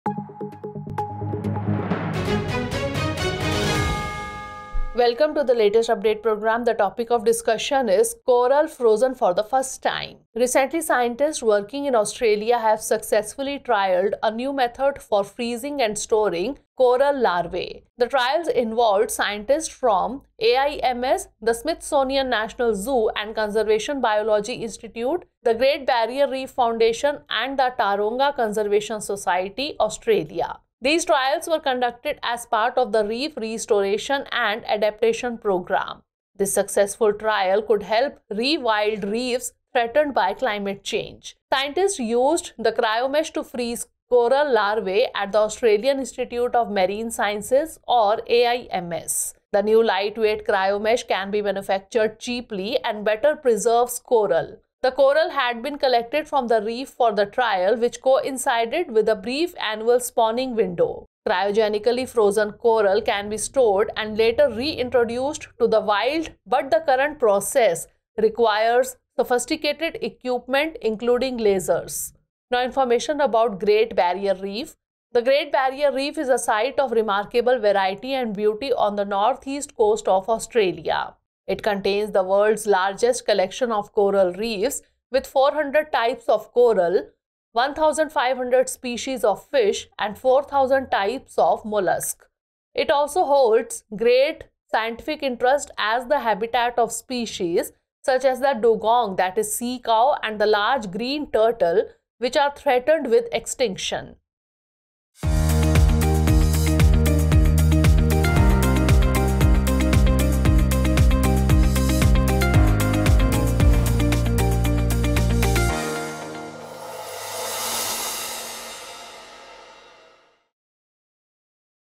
Coral Frozen For The First Time Latest update | Drishti IAS English
Recently, Scientists working in Australia have successfully trialed a new method for freezing and storing coral larvae. These trials were conducted as part of the Reef Restoration and Adaptation Program.